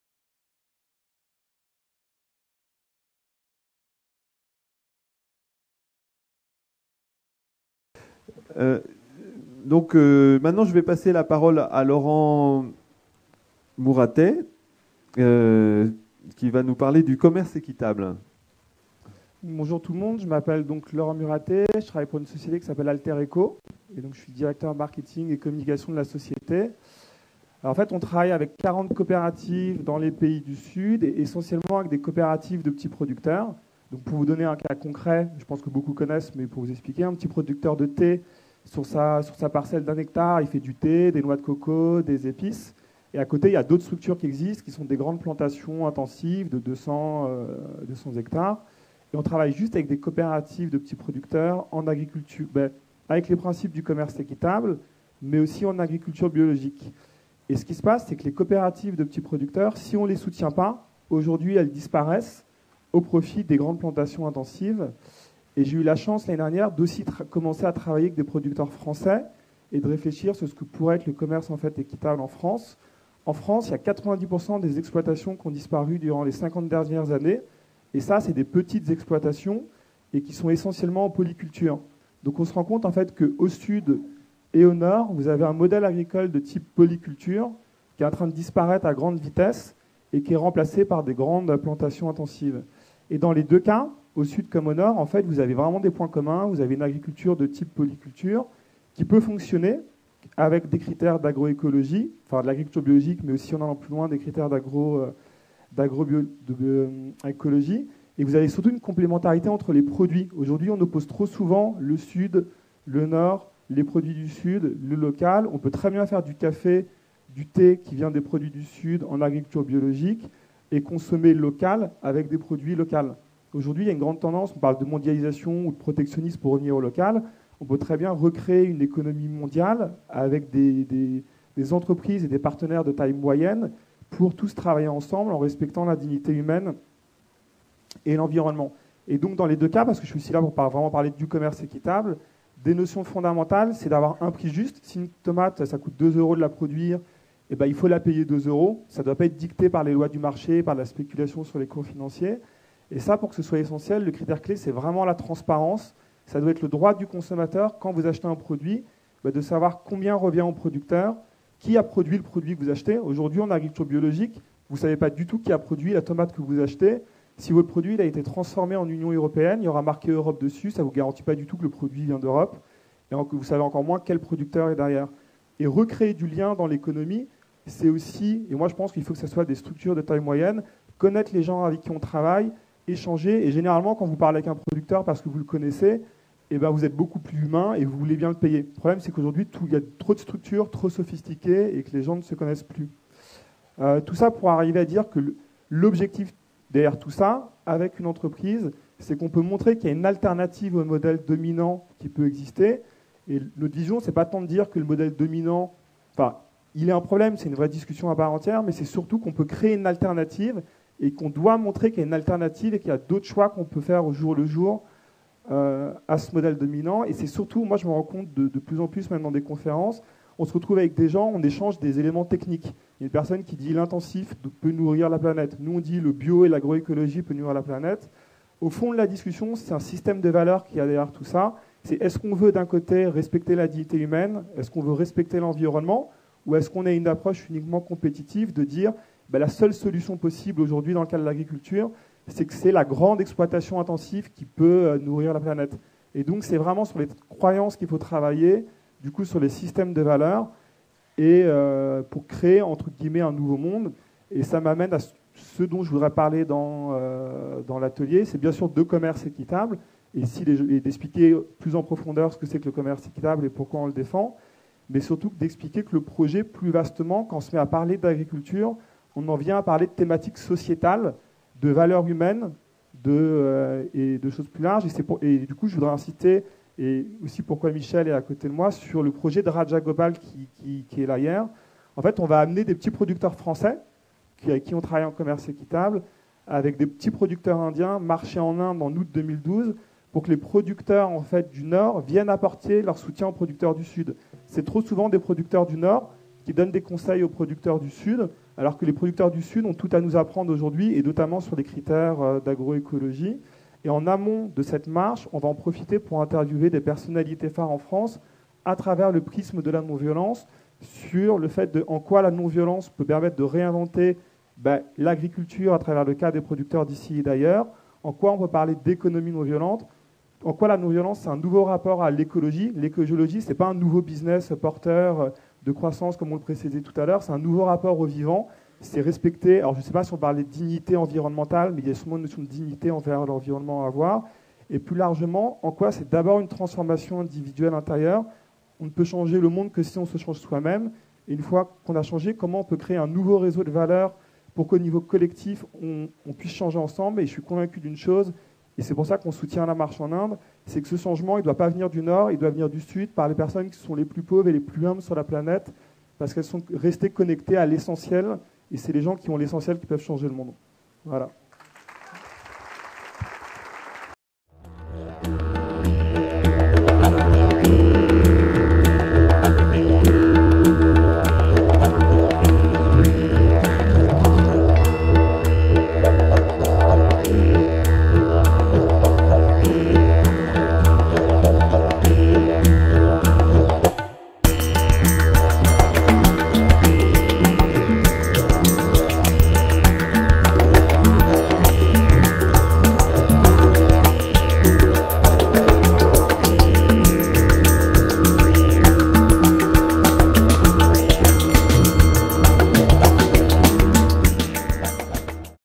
Alter Eco au Forum économie et spiritualité - Institut Karmaling
Laurent Muratet, Directeur Marketing et Communication d'Alter Eco explique la mission d'Alter Eco au sud mais également au nord ainsi que leur soutien à des initiatives comme celle de la grande marche non-violente d'Ekta Parishad prévue en Inde et qui durera un an à partir du 2 octobre 2011.